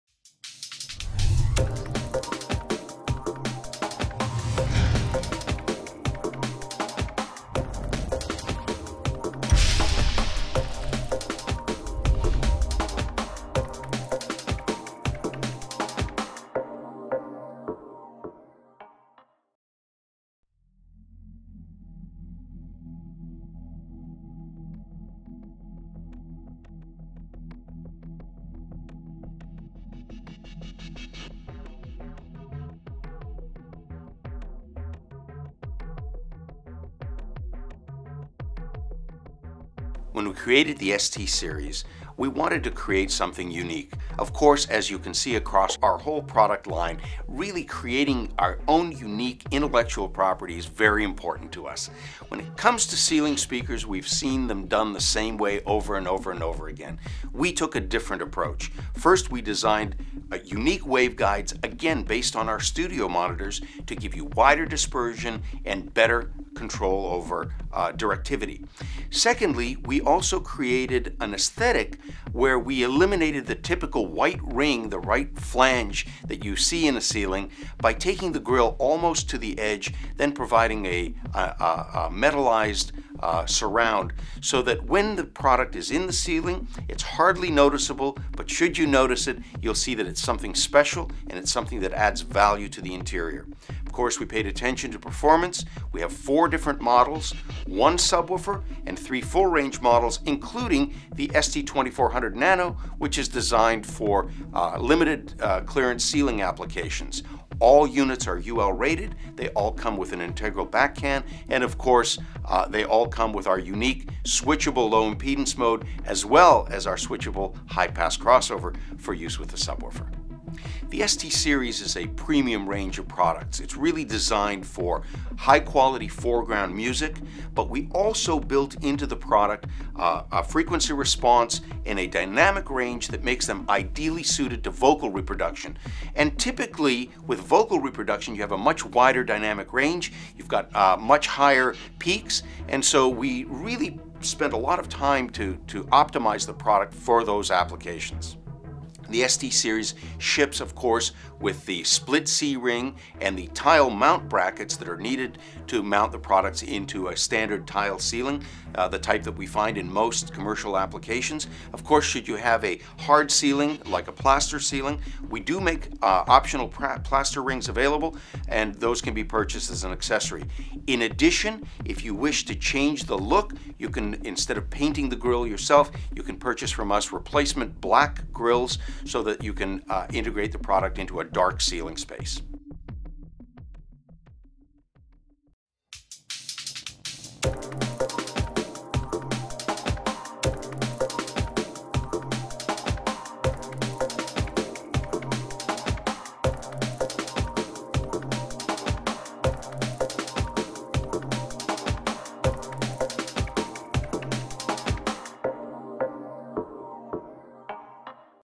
EUROCOM ST Series In-Ceiling Speakers
ST Series loudspeakers are unlike any in-ceiling speaker you've seen-or heard! With our exceptional aesthetics and premium-grade sound, the ST Series features a proprietary range of concentric drivers that deliver exceptionally broad frequency response and ultra-wide dsipersion; all manufactured to exacting quality control standards in our BEHRINGER manufacturing facility. Offered in a variety of full-range and complementary subwoofer models, the EUROCOM ST Series offers unmatched performance, style and value.